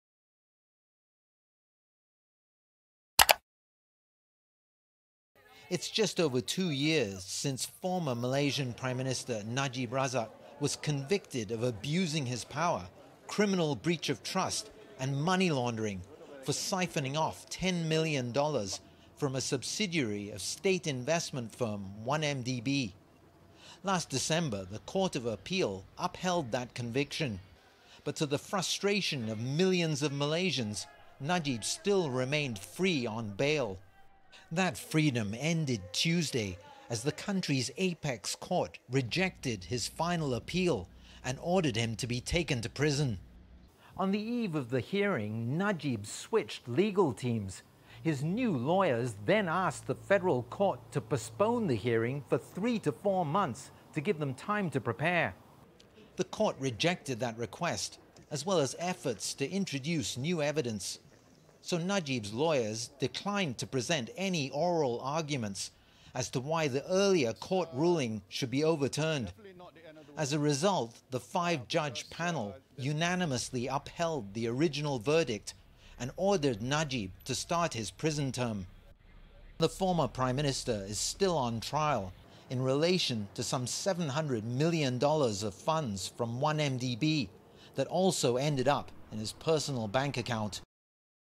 Malaysia ex-PM Najib begins 12-year jail sentence
Its just over two years since former Malaysian prime minister Najib Razak was convicted of abusing his power, criminal breach of trust and money laundering. The country’s apex court rejected his final appeal and ordered him to be taken to prison. The five judge panel unanimously upheld the original verdict and ordered the ex-prime minister to start his prison term.